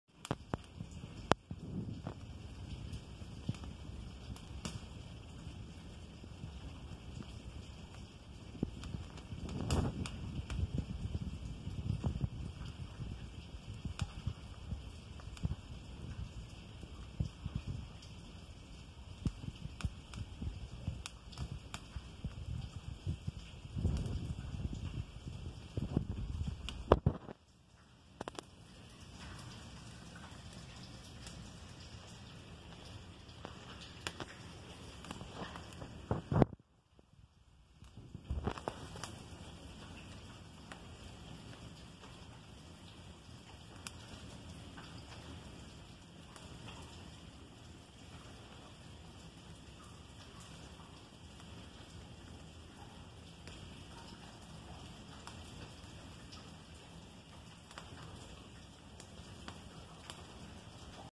Campfire in a cave
I wanted to include this as short in the cave video, along with the next one I'm uploading, but video editing software has become awfully scarce in modern times. I had a 15 year old laptop that at least had windiws movie maker. Now I buy one brand new and can't even download a program that works for cheap. Talk about regression, what a joke silicon valley is. Oh well, here's the clip.